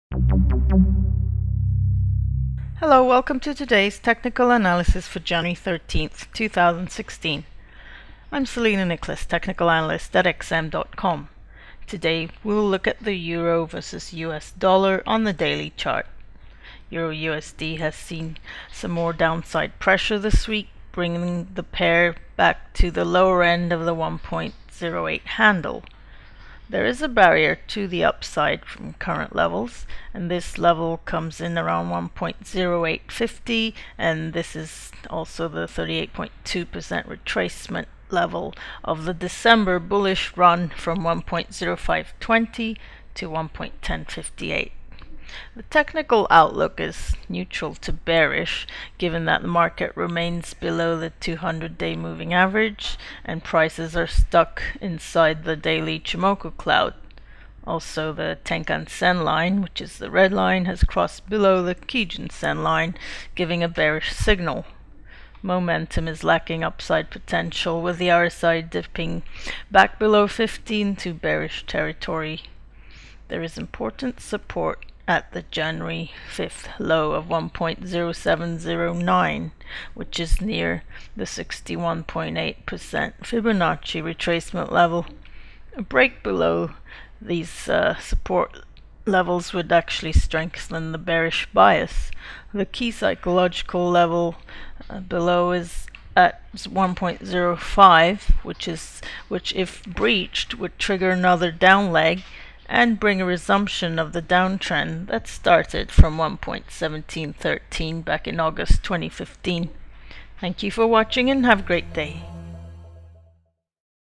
Technical Analysis - 13/01/2016 - EURUSD sees more downside pressure
Daily Technical Analysis of the Forex Markets - to guide your trading using simple technical indicators such as moving averages, RSI, Fiboancci, Ichimoku.